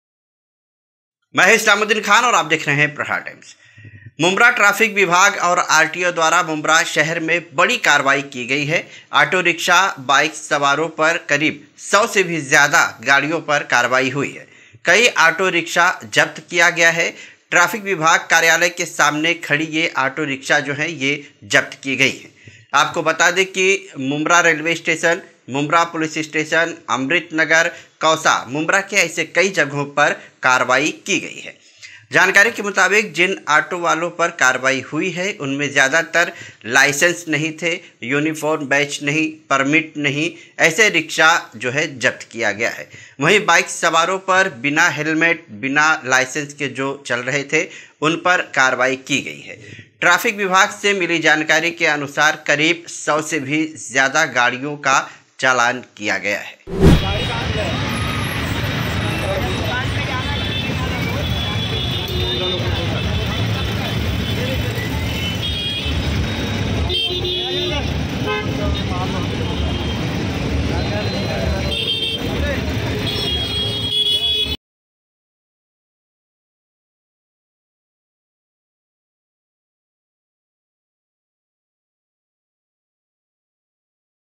मुम्ब्रा ट्राफ़िक और RTO ने 100 से ज्यादा गाड़ियो पर कार्यवाई || Islamuddin Khan|| Aapka Prahar Times
मुम्ब्रा ट्राफ़िक और RTO ने 100 से ज्यादा गाड़ियो पर कार्यवाई || Islamuddin Khan

मुंब्रा ट्राफ़िक विभाग और RTO द्वारा  मुंब्रा शहर में बड़ी कार्यवाई की गई ऑटो रिक्शा, बाइक सवारों पर करीब 100 से भी ज्यादा गाड़ियो पर कार्यवाई हुई है,,कई ऑटो रिक्सा जप्त किया गया ट्रैफिक विभाग के सामने खड़ी ये ऑटो रिक्शा जप्त किया गया है ,,आपको बतादे की मुंब्रा रेलवे स्टेशन, मुंब्रा पुलिस स्टेशन,अमृत नगर,कौसा ऐसे कई जगहों पर कार्यवाई हुई है जानकरी के मुताबिक जिन ऑटो वालो पर कार्यवाई हुई उनमें ज्यादातर लाइसेंस नही थे यूनिफॉर्म बैच नही,परमिट नही थे ऐसे रिक्शा जप्त किया गया वही बाइक सवारों पर बिना हलेमेट,बिना लाइसेंस के जो चल रहे थे उनपर कार्यवाई की गई है ट्राफ़िक विभाग से मिली जानकारी के अनुसार 100 से भी ज्यादा गाड़ियो का चलान किया गया